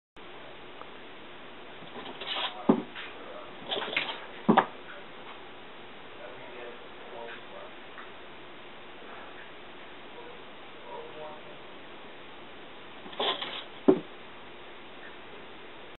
Kitty High Jump
One of my cats is trying to get a tiny bug that is near the top of the door.